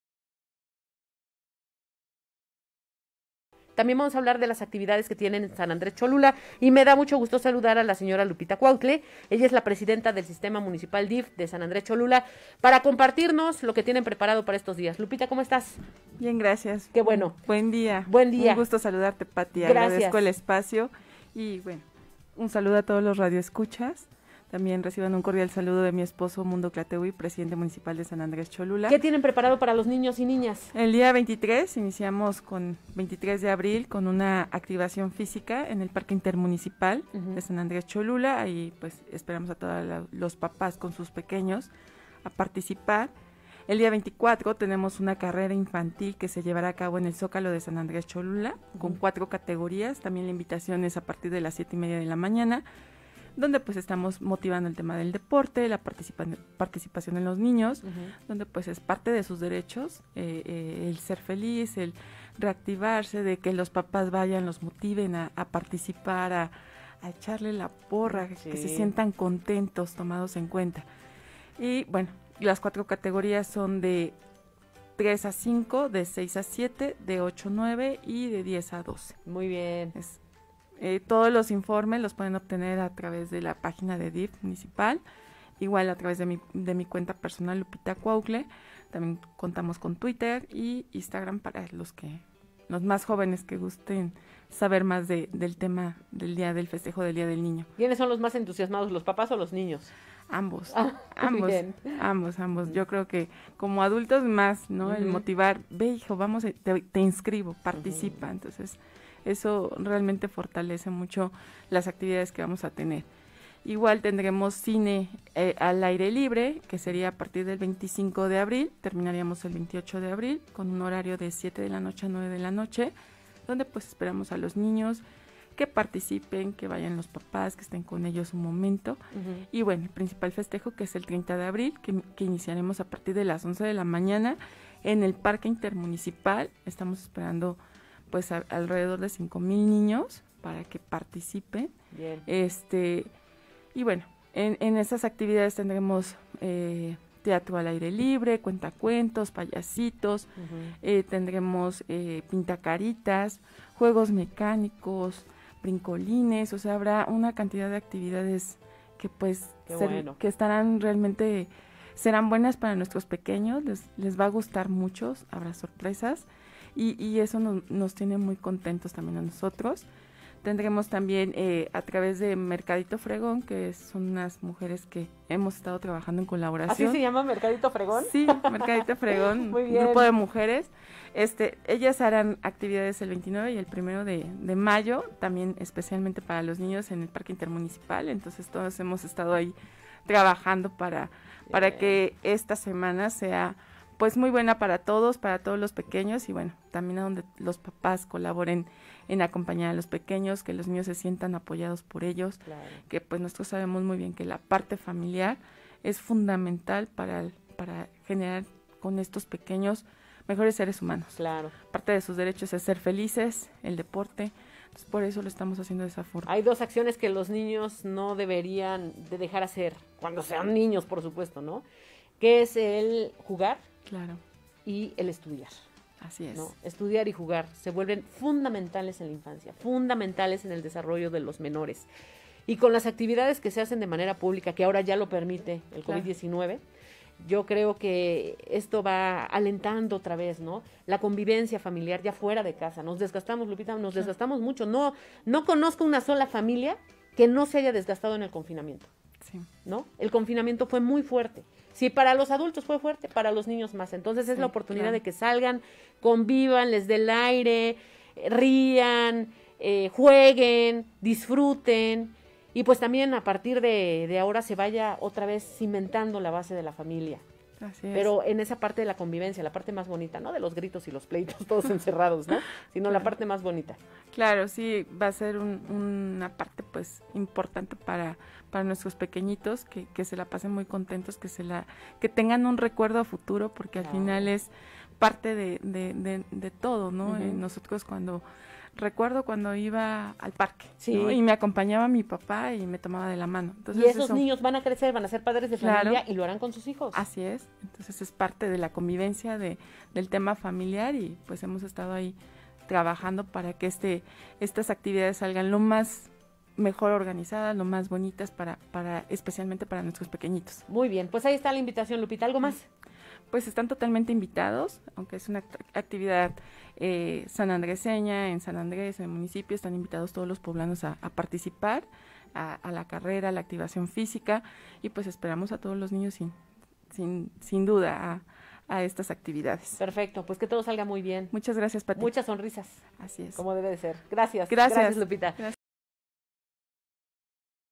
Anuncia Lupita Cuautle festejos del Día del Niño en San Andrés Cholula
Lupita Cuautle, Presidenta del Patronato y Voluntariado del Sistema DIF Municipal de San Andrés Cholula, invitó a los niños y sus padres a festejar el Día del Niño con las actividades que realizarán en el municipio.

En entrevista con Patricia Estrada, Cuautle informó que del 23 al 30 de abril realizarán las siguientes actividades para los festejos del Día del Niño.

Las actividades arrancarán el próximo 23 de abril con una activación física en el Parque Intermunicipal de San Andrés Cholula.